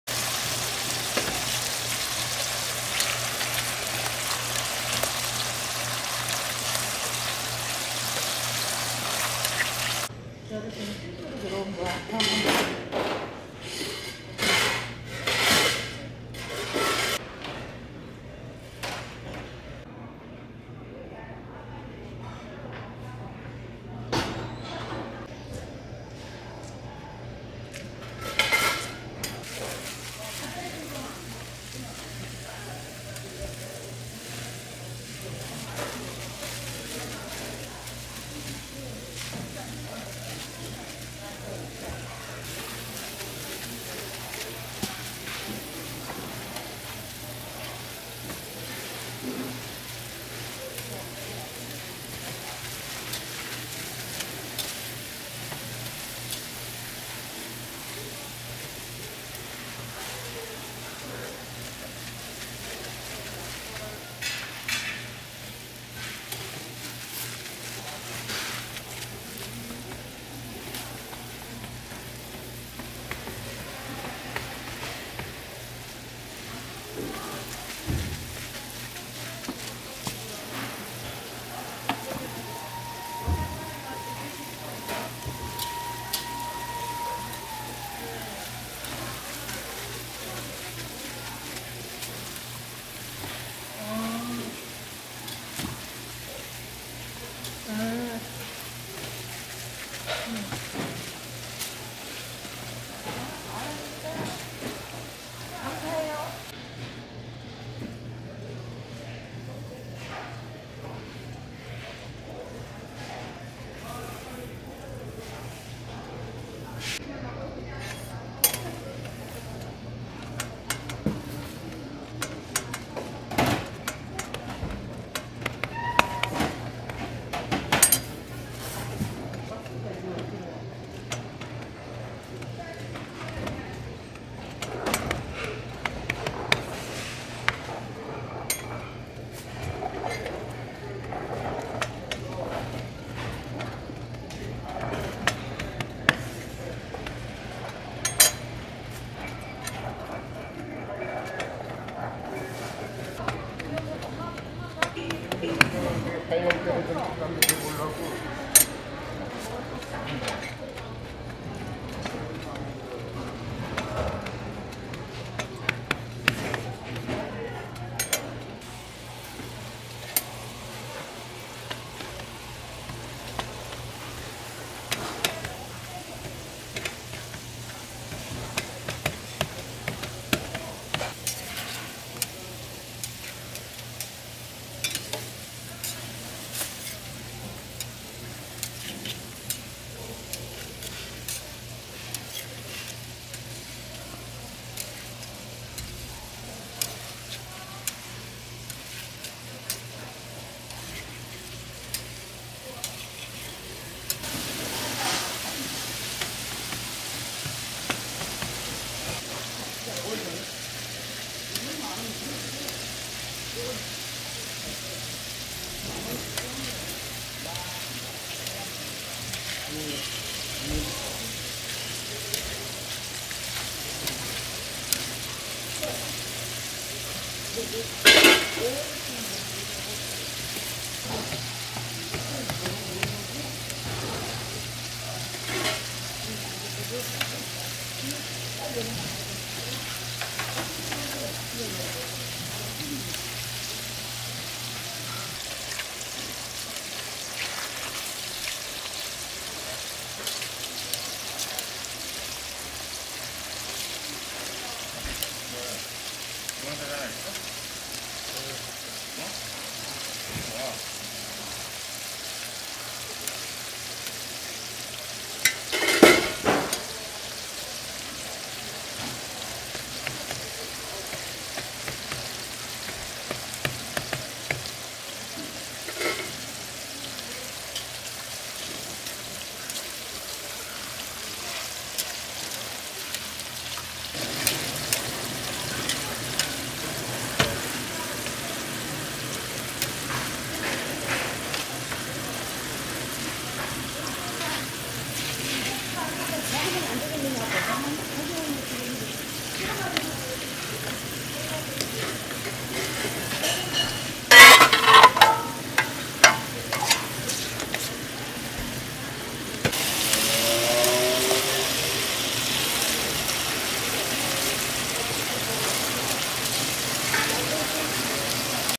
서민갑부 육즙 가득 수제 떡갈비! | 우림시장 길거리음식 Meatballs, Korean street food
우림시장 1982떡갈비 냠냠

✔️WORLD STREET FOOD AND RESTAURANT

✔️ 아무거나 다 주워 먹습니다만
맛있는건 맛있다  맛없는건 맛없다 확실하게 표현합니다!

✔️선택은 여러분의 몫 입니다